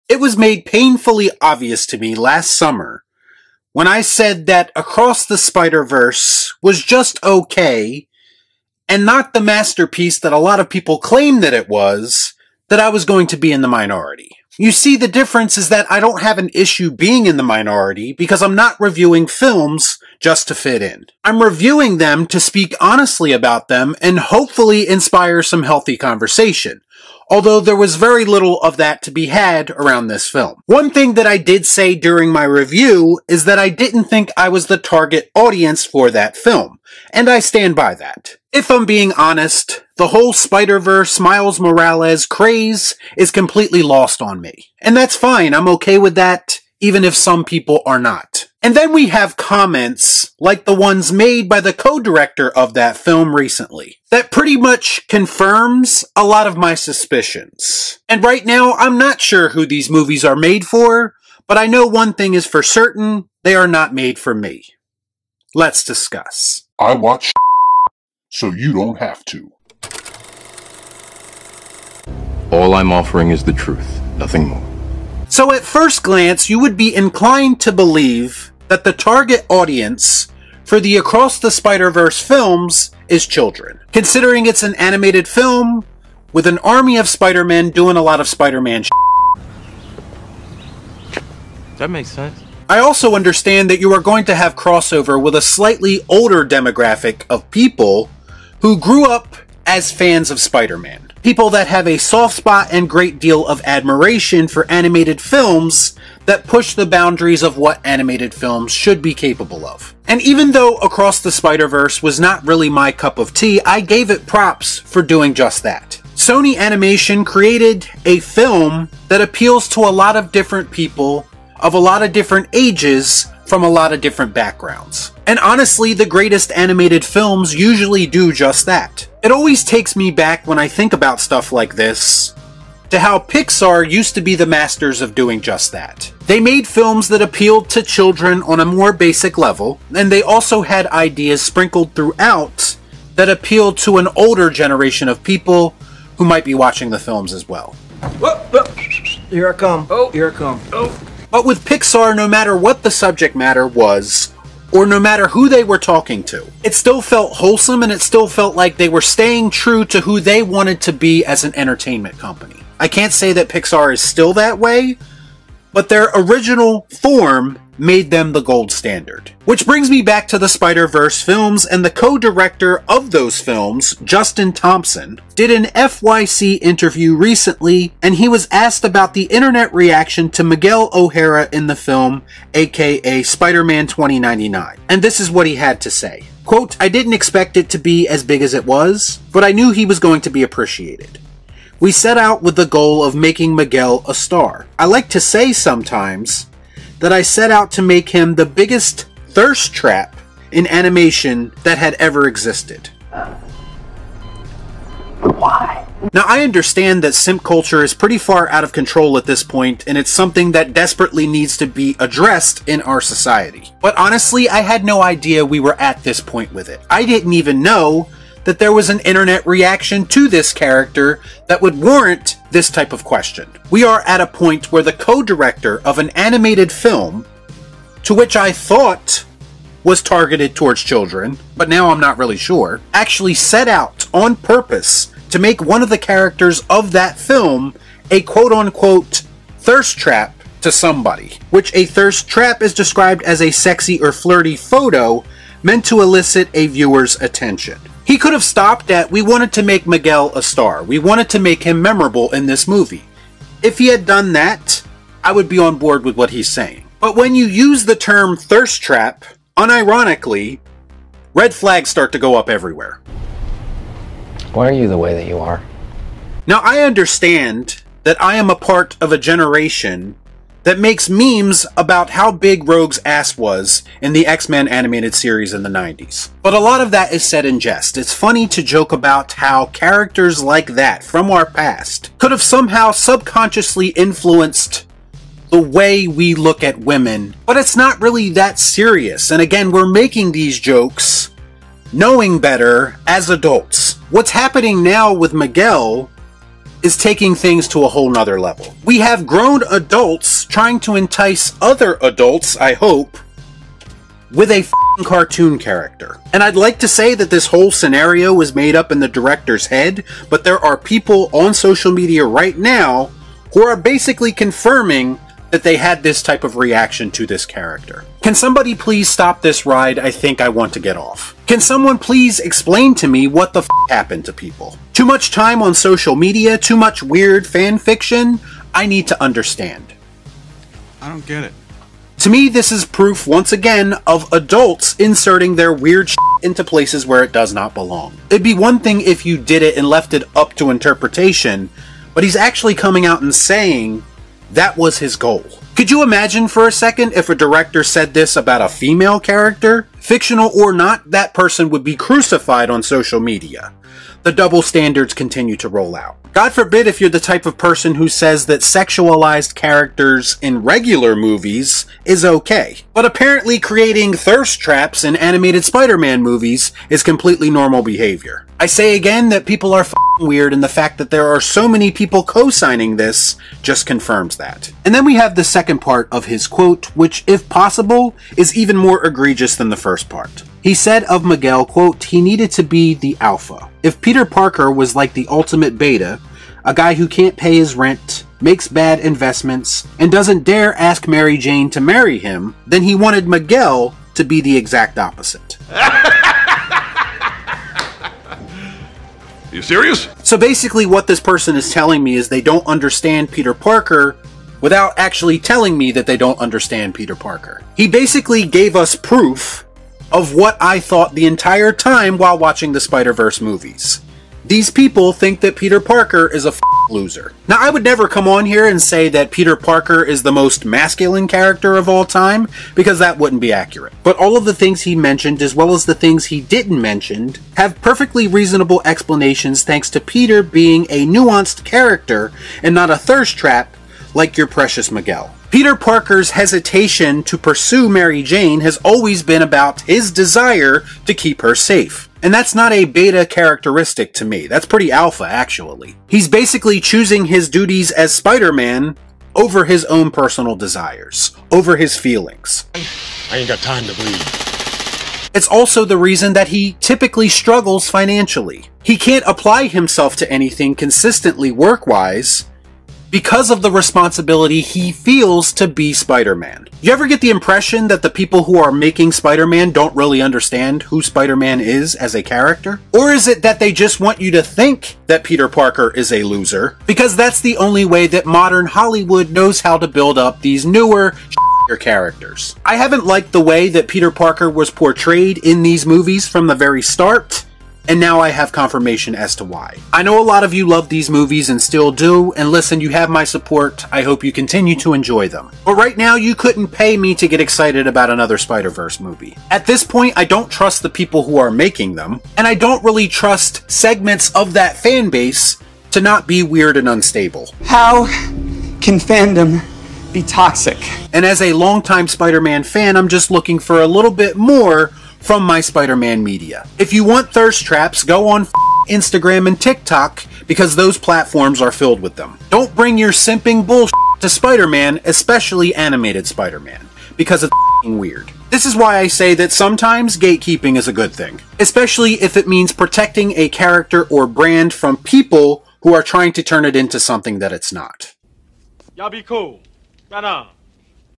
SPIDERVERSE DIRECTOR HATES "BETA LOSER" PETER PARKER & LOVES "THIRST TRAP" MIGUEL? | REEL SHIFT
My reaction to Across the Spiderverse director saying he wanted Miguel to be a "thirst trap" & how Peter Parker is the ultimate Beta

MY MOVIE RATING SYSTEM:
Masterpiece - Make Em' An Offer